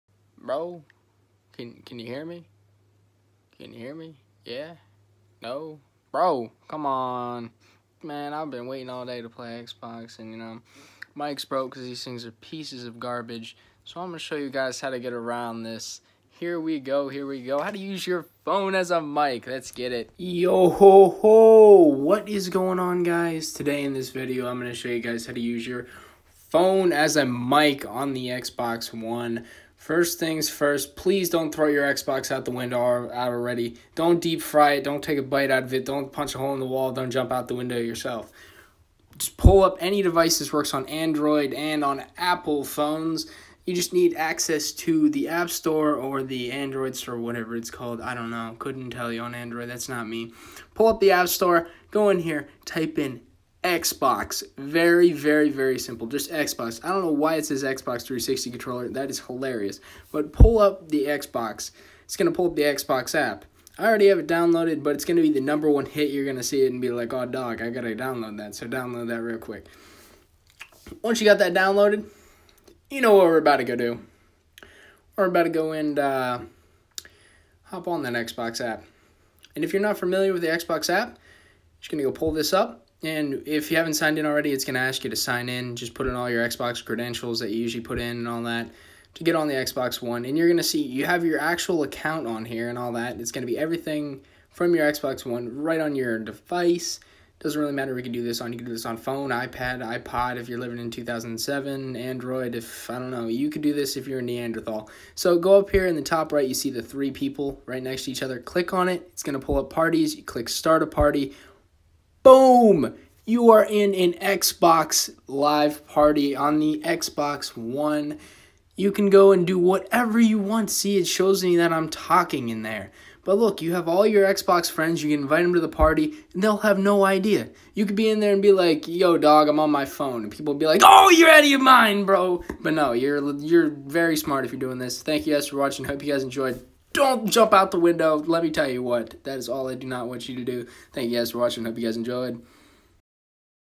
"How To Use Your Phone As A Mic On Xbox One" - Use Any Phone As A Mic To Chat On Xbox One (Easy)
In this video I show you "How To Use Your Phone As A Mic On The Xbox one". This is very simple and "you can use any phone as a mic on the xbox one".